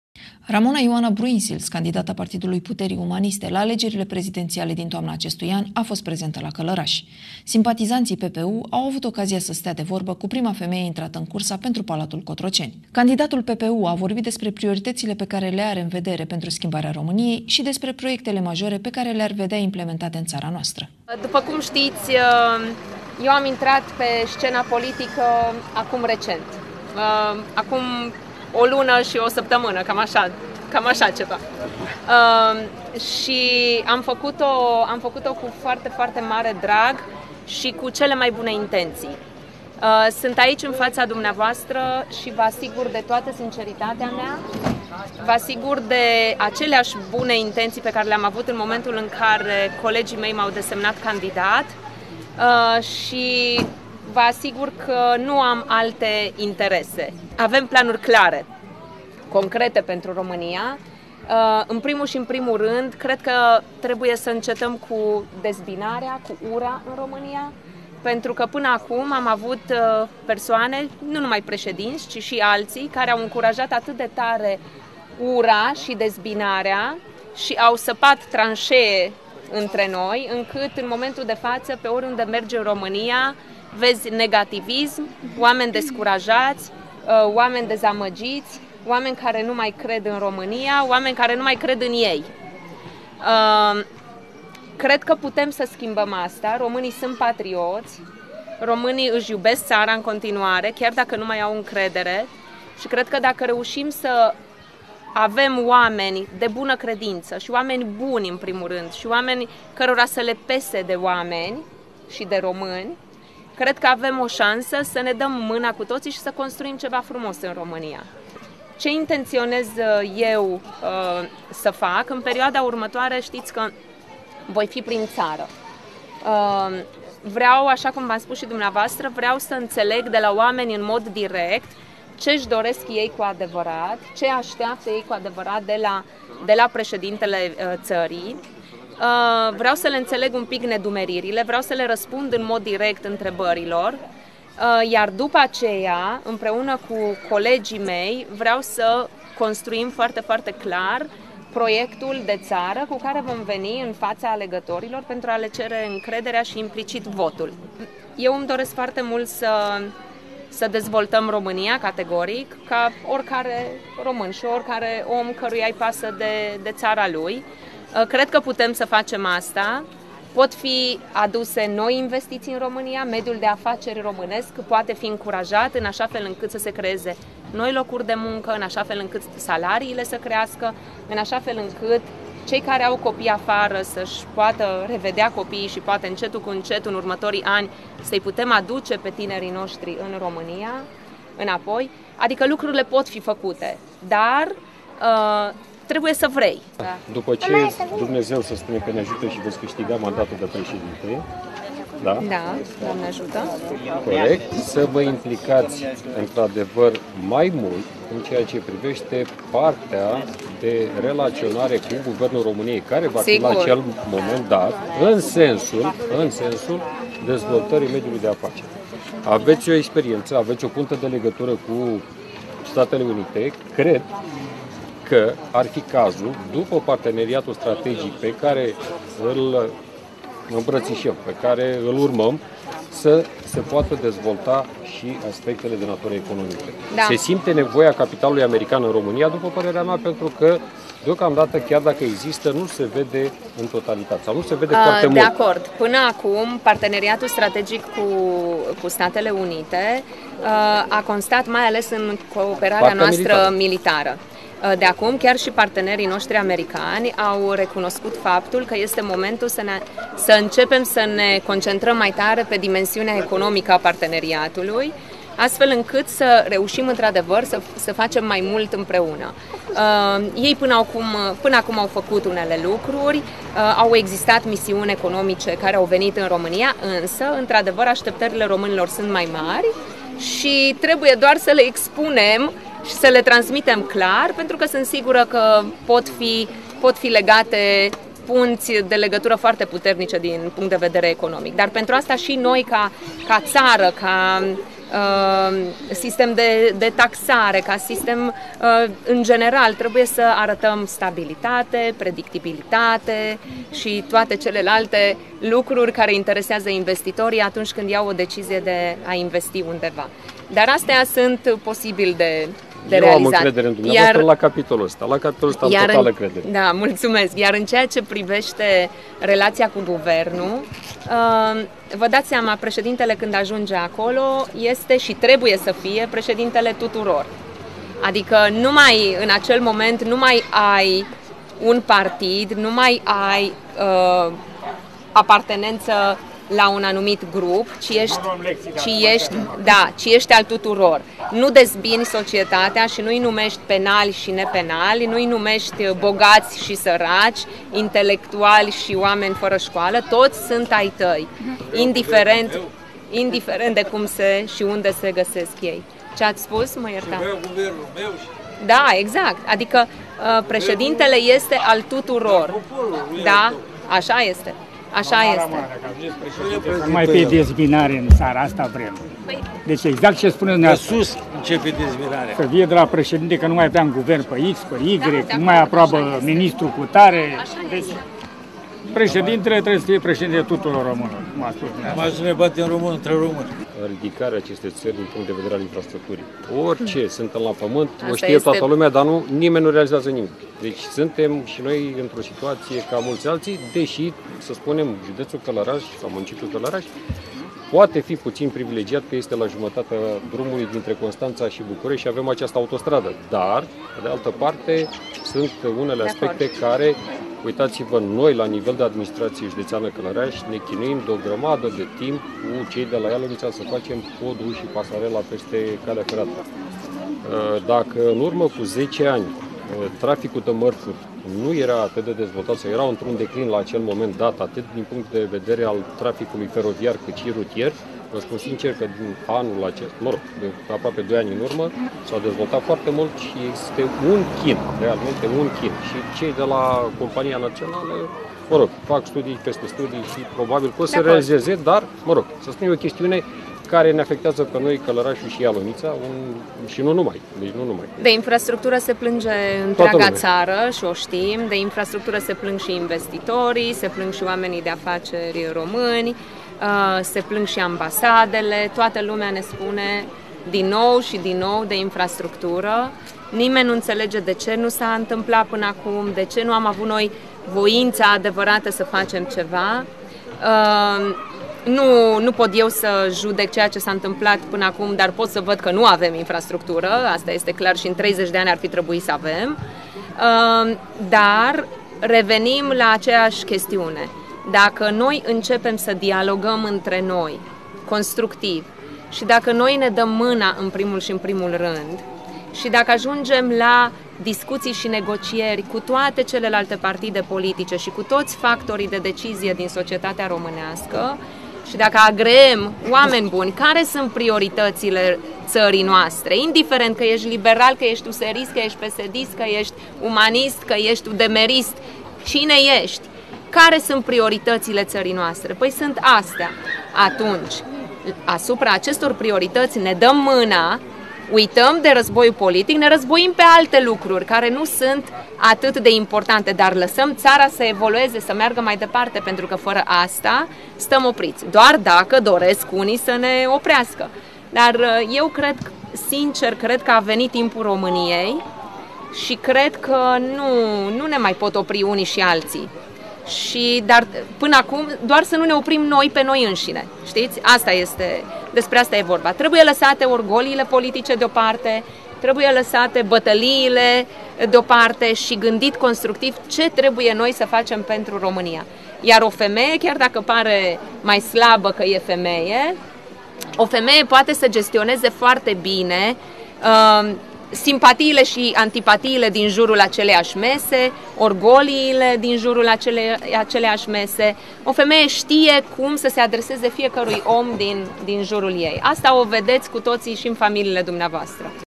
Știri Locale - Ramona Bruynseels în Mun. Călărași
Știri Locale

Ramona Ioana Bruynseels, candidata PPU la alegerile prezidențiale din toamna acestui an, a fost prezentă la Călărași.